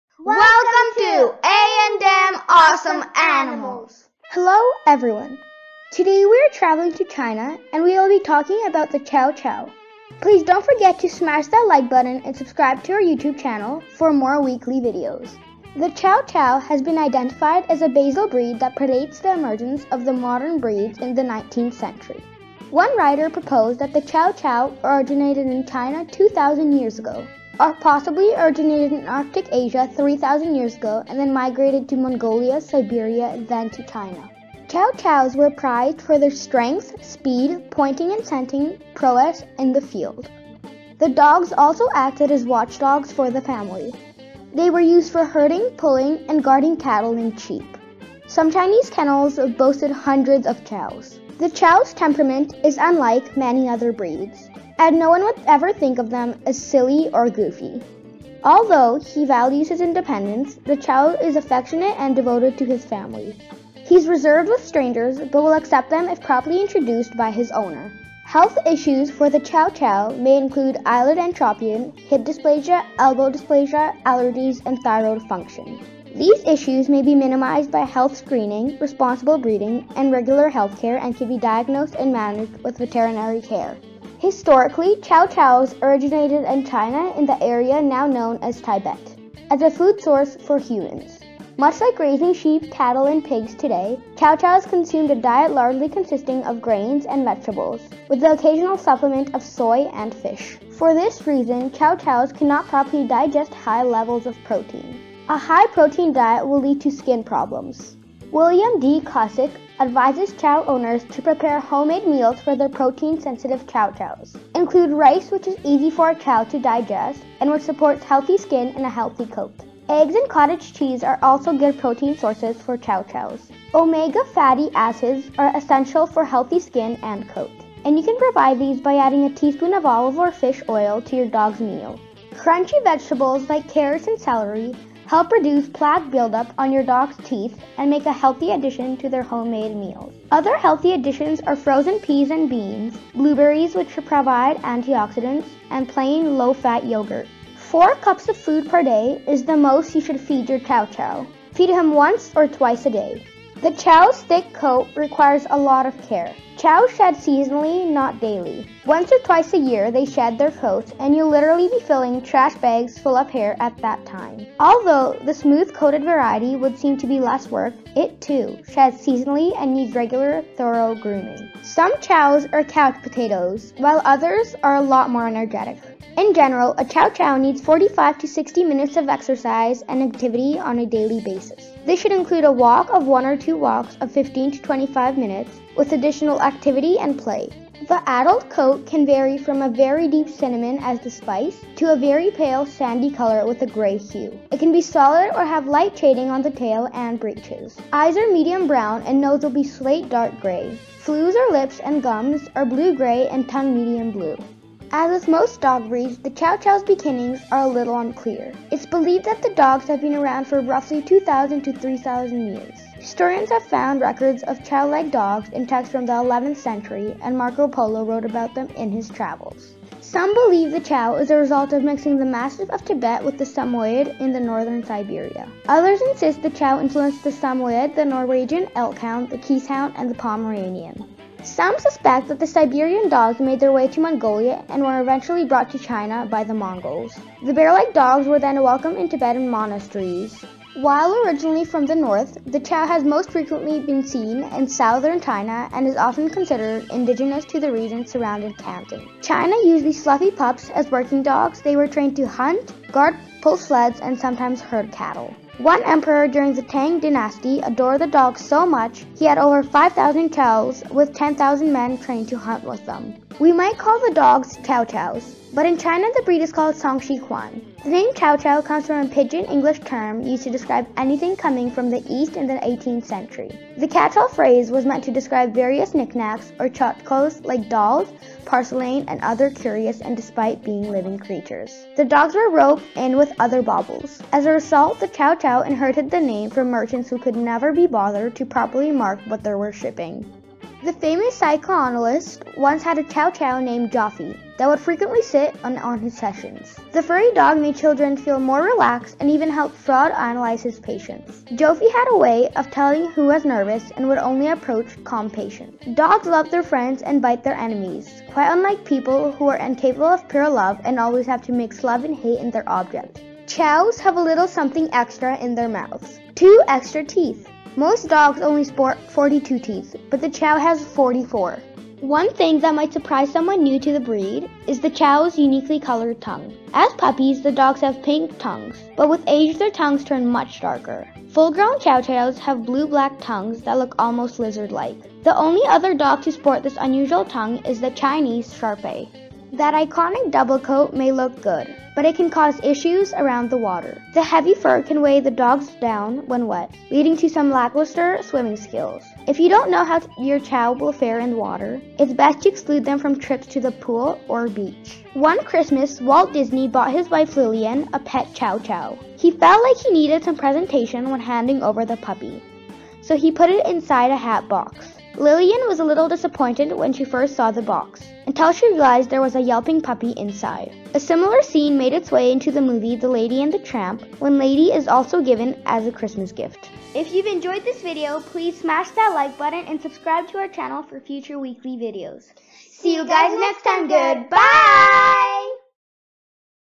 Chow Chow - The Best Gift
Historians have found records of a chow-like dog in texts from the 11th century, and Marco Polo wrote about them in his travels. Some believe the chow is a result of mixing the mastiff of Tiber with the Samoyed in Northern Siberia. Others insist the chow influenced the Samoyed, the Norwegian elkhound, the keeshond, and the Pomeranian. While originally from the North, the chow chow has most frequently been seen in Southern China and is often considered indigenous to the region surroundings Canton... Please watch this video to learn interesting facts about chow chow!